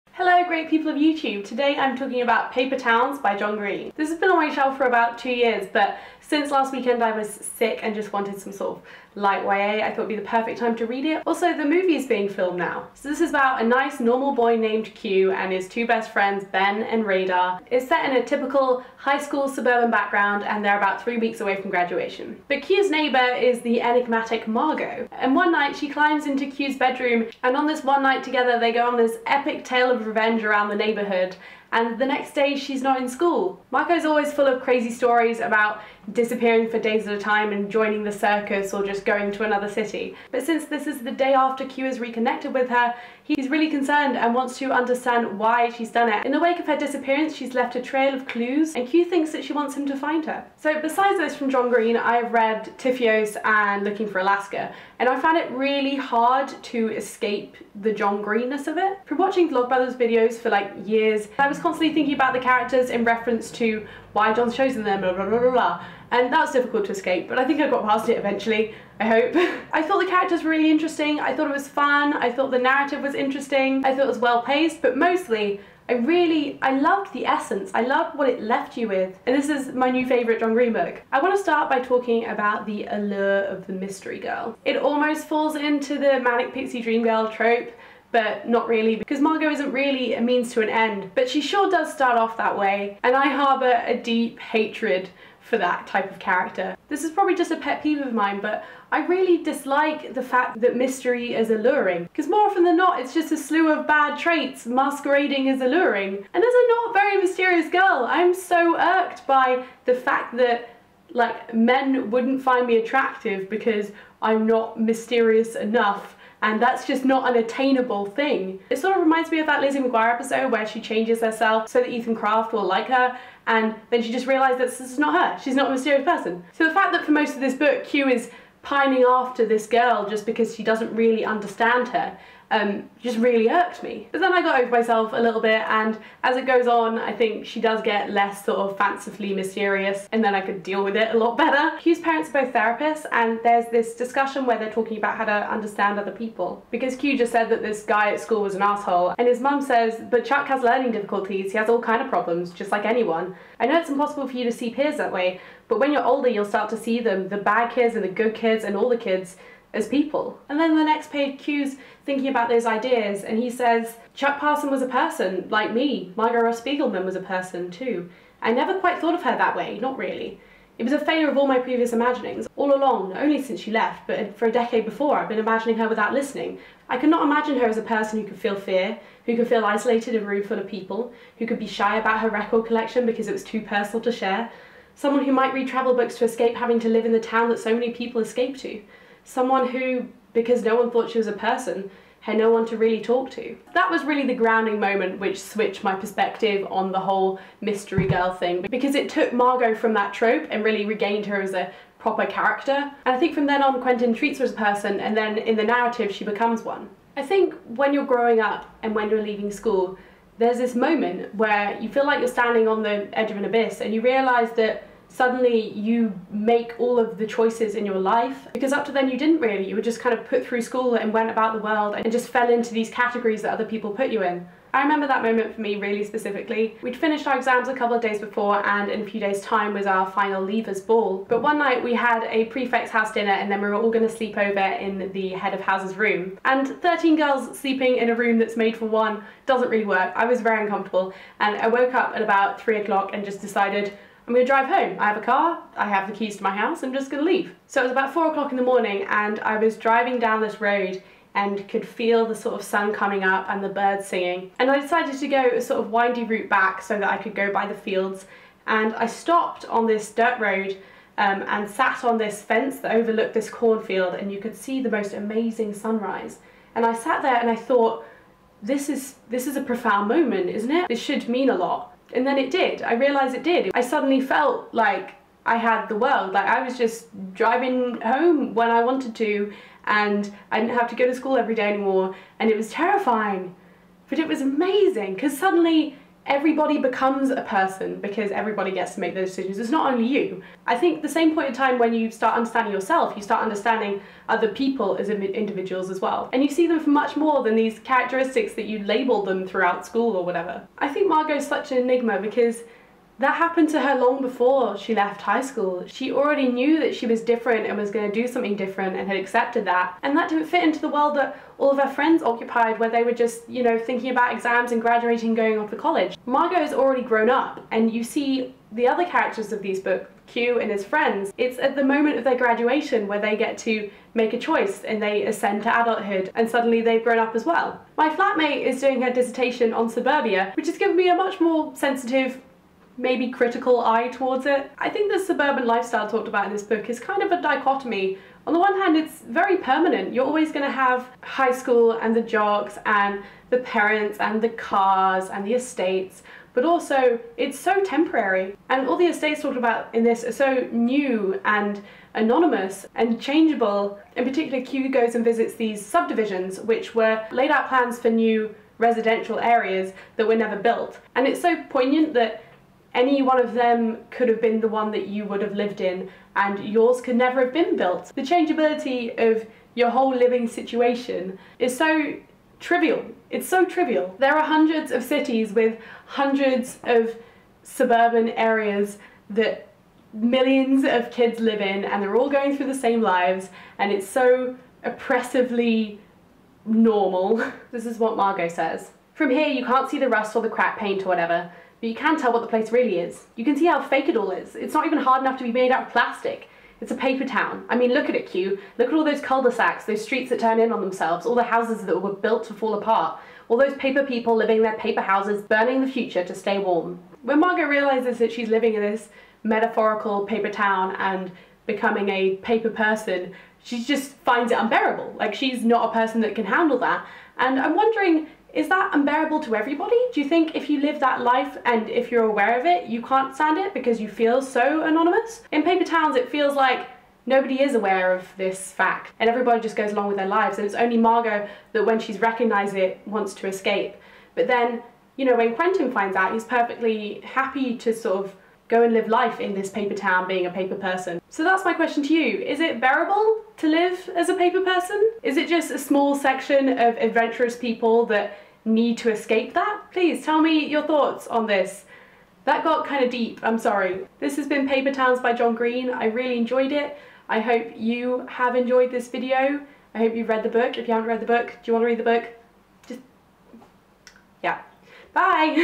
Paper Towns by John Green
This is long but worth it.

Summary 0:13
Escaping John Green 0:50
Review 1:09
The Mystery Girl 1:24
Growing Up 3:26
Suburbia, Paper Towns 5:28

This thumbnail took 2 hours. You better like it.